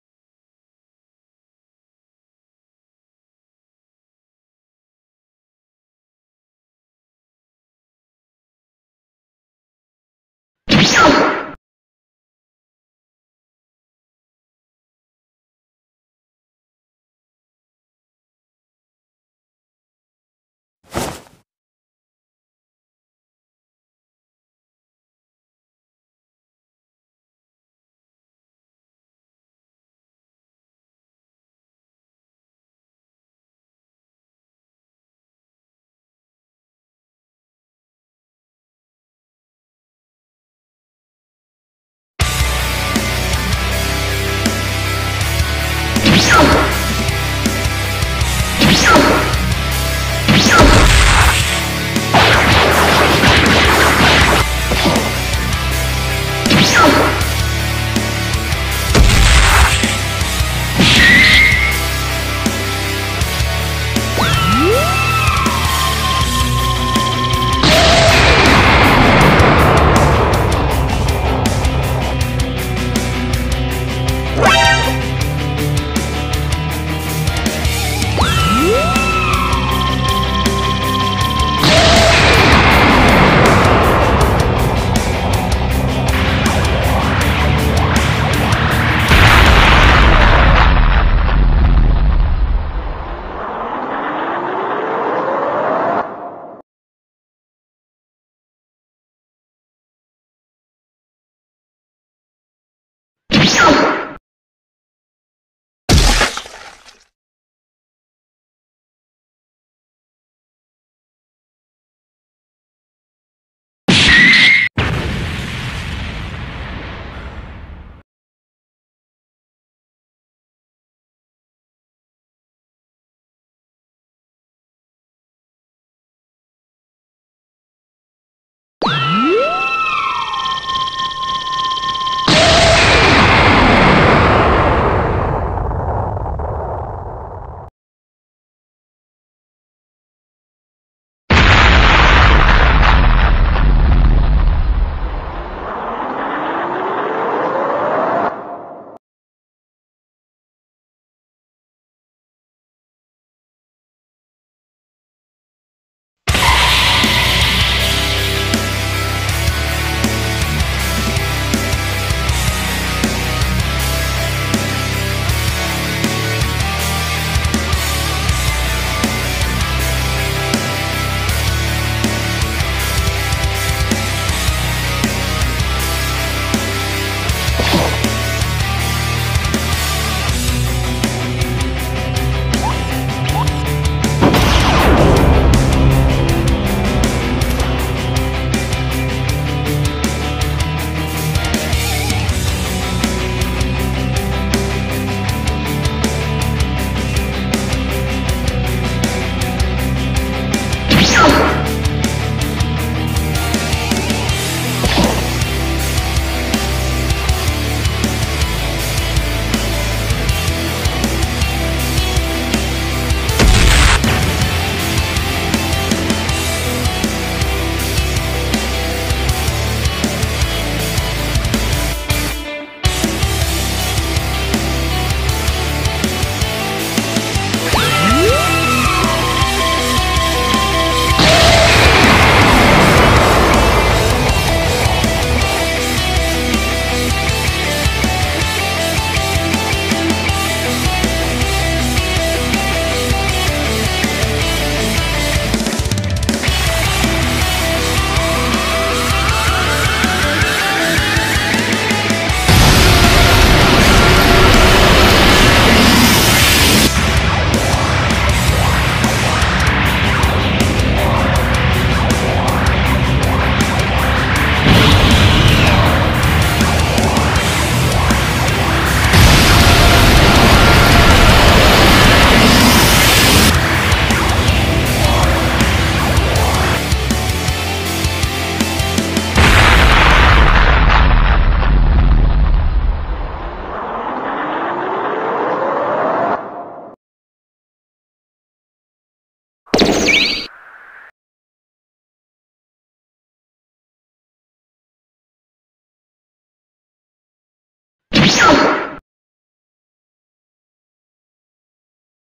Classic Caillou's Return, (DBZ Gacha club)
That's right guys, classic Caillou Is now back,
but stronger than ever,